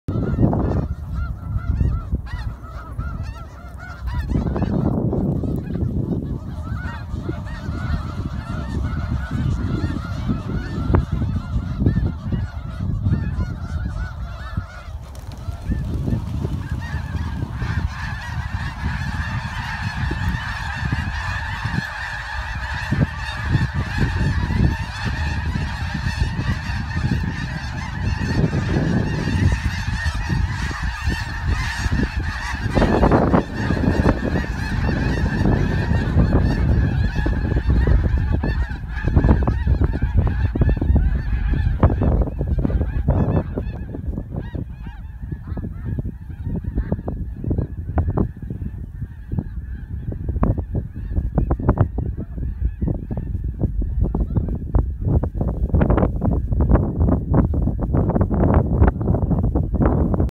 SaraStamey Snow Geese Skagit Valley 2020
Every winter, huge flocks of snow geese browse the muddy fields of Skagit Valley in Northwest Washington state.